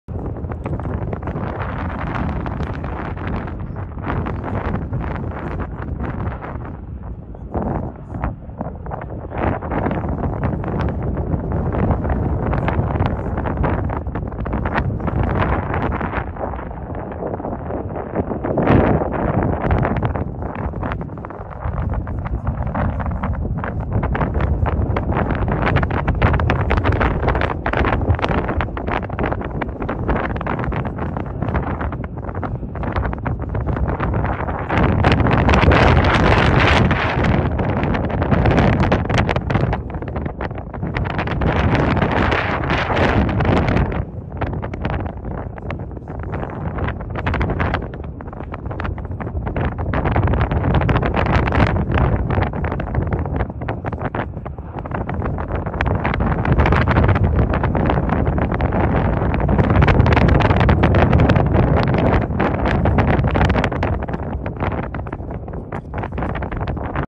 LAVA Breakout from main Pond at Sundhnukur, April 2, 2024 (with Wind Sound)
Some folks said they did not like the music. Thats fine...here the wind sound of the original clip.
Video from James Day